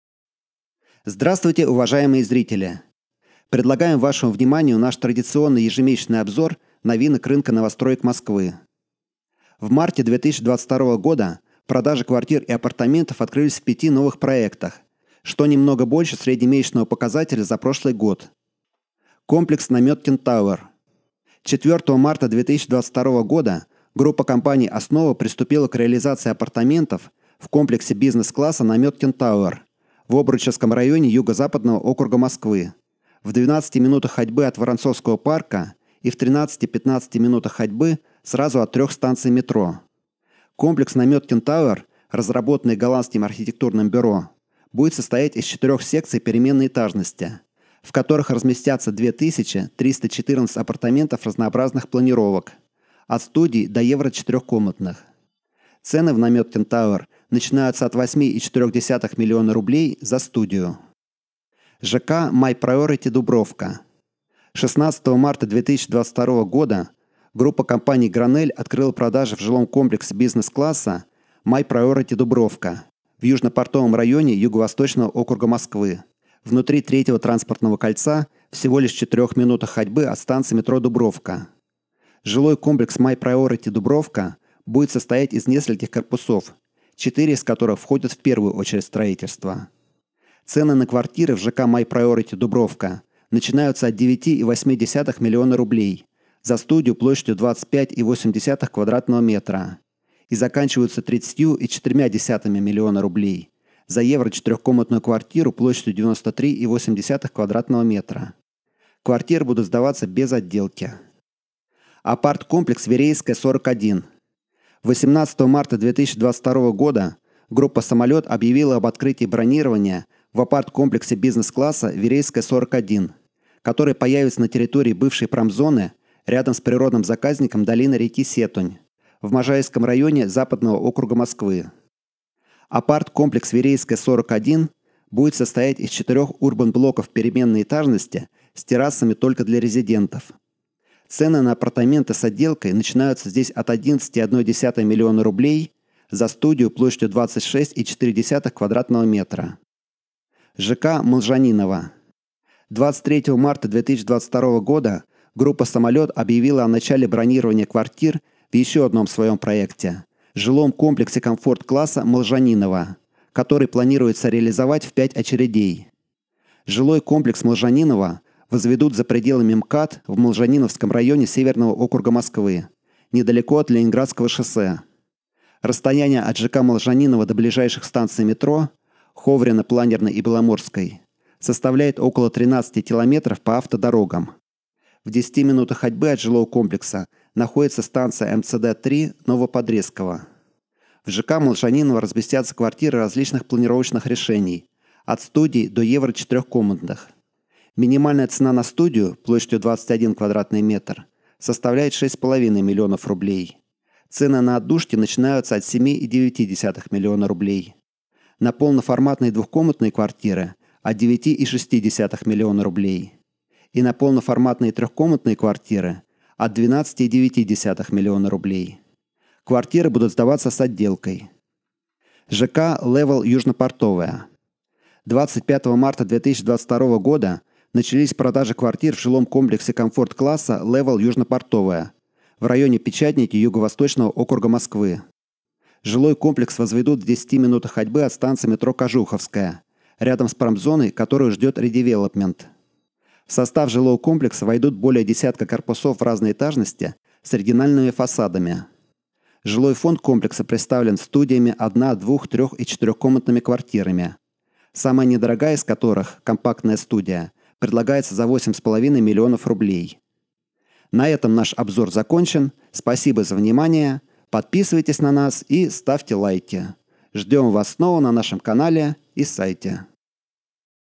Старты продаж в новостройках Москвы в Марте 2022 года
В марте 2022 года продажи квартир и апартаментов открылись в пяти новых проектах: